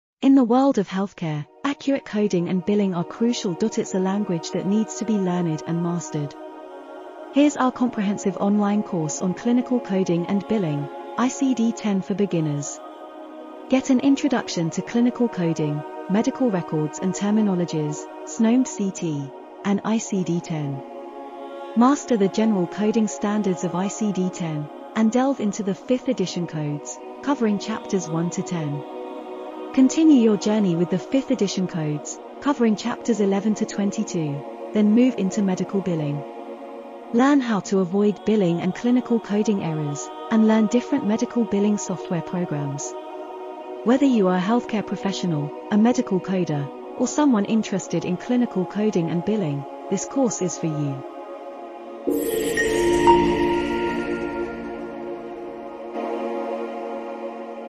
Clinical Coding | Learndrive | Online Course Trailer
Unlock the secrets of medical data with "Clinical Coding | Learndrive | Online Course Trailer."

Course Highlights:
✅ Understand the fundamentals of clinical coding
✅ Learn key terminologies and medical classifications
✅ Get hands-on experience with coding software
✅ Acquire skills to accurately translate medical records into codes
✅ Gain insights into the ethical and legal considerations of clinical coding

Ideal for healthcare professionals, administrators, and students interested in medical data and informatics, this course will equip you with the necessary skills to thrive in a clinical coding environment.

📊 Be the bridge between healthcare providers and data analysts. Master the art of Clinical Coding today!